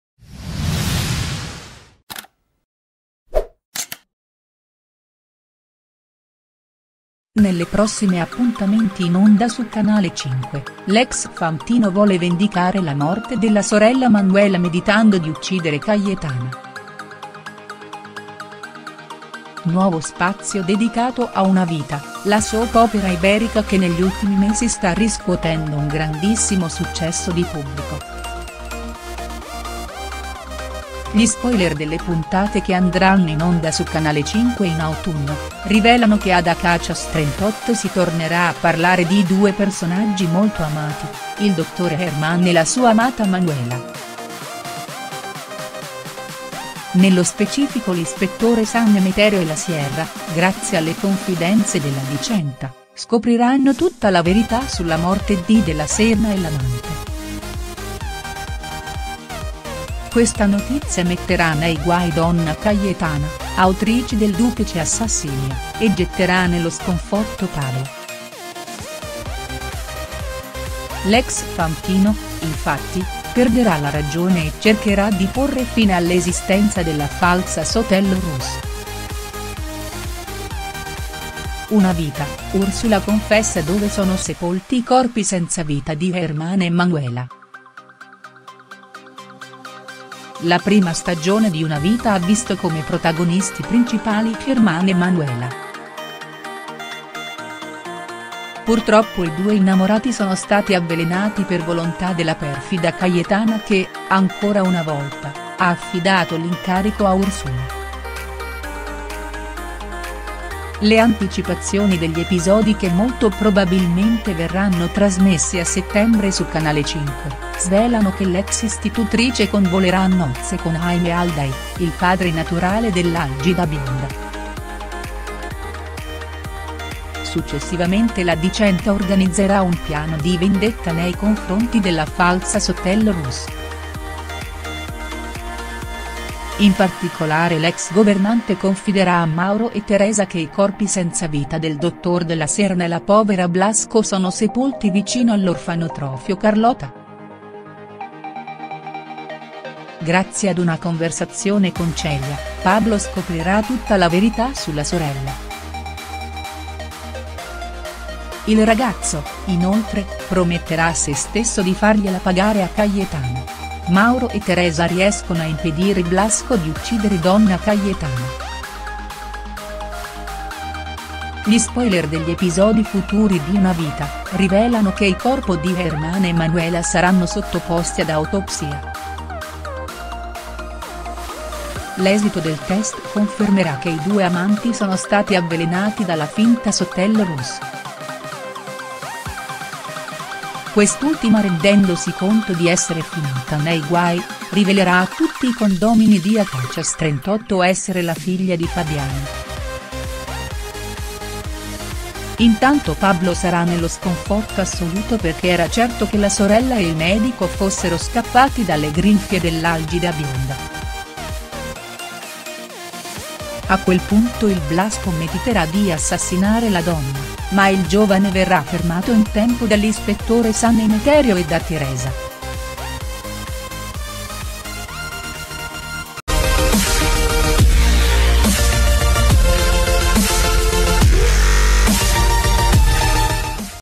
Anticipazioni Una Vita, puntate italiane: Paolo Blasco vuole assassinare donna Cayetana
Nelle prossime appuntamenti in onda su Canale 5, l'ex fantino vuole vendicare la morte della sorella Manuela meditando di uccidere Cayetana. Nuovo spazio dedicato a 'Una Vita' , la soap opera iberica che negli ultimi mesi sta riscuotendo un grandissimo successo di pubblico. Gli spoiler delle puntate che andranno in onda su Canale 5 in autunno, rivelano che ad Acacias 38 si tornerà a parlare di due personaggi molto amati: il dottore German e la sua amata Manuela. Nello specifico l'ispettore San Emeterio e la Sierra, grazie alle confidenze della Dicenta, scopriranno tutta la verità sulla morte di De La Serna e l'amante. Questa notizia metterà nei guai donna Cayetana (autrice del duplice assassinio) e getterà nello sconforto Pablo. L'ex fantino, infatti, perderà la ragione e cercherà di porre fine all'esistenza della falsa Sotelo Ruz. Una Vita: Ursula confessa dove sono sepolti i corpi senza vita di German e Manuela. La prima stagione di 'Una vita' ha visto come protagonisti principali German e Manuela. Purtroppo i due innamorati sono stati avvelenati per volontà della perfida Cayetana che, ancora una volta, ha affidato l'incarico a Ursula. Le anticipazioni degli episodi che molto probabilmente verranno trasmessi a settembre su Canale 5, svelano che l'ex istitutrice convolerà a nozze con Jaime Alday, il padre naturale dell'algida bionda. Successivamente la Dicenta organizzerà un piano di vendetta nei confronti della falsa Sotelo Ruz. In particolare l'ex governante confiderà a Mauro e Teresa che i corpi senza vita del dottor De La Serna e la povera Blasco sono sepolti vicino all'orfanotrofio 'Carlota'. Grazie ad una conversazione con Celia, Pablo scoprirà tutta la verità sulla sorella. Il ragazzo, inoltre, prometterà a sé stesso di fargliela pagare a Cayetana. Mauro e Teresa riescono a impedire Blasco di uccidere donna Cayetana. Gli spoiler degli episodi futuri di 'Una Vita' , rivelano che i corpo di German e Manuela saranno sottoposti ad autopsia. L'esito del test confermerà che i due amanti sono stati avvelenati dalla finta Sotelo Ruz. Quest'ultima rendendosi conto di essere finita nei guai, rivelerà a tutti i condomini di Acacias 38 essere la figlia di Fabiana. Intanto Pablo sarà nello sconforto assoluto perché era certo che la sorella e il medico fossero scappati dalle grinfie dell'algida bionda. A quel punto il Blasco mediterà di assassinare la donna, ma il giovane verrà fermato in tempo dall'ispettore San Emeterio e da Teresa. I due convinceranno l'ex fantino di attendere la sentenza del giudice.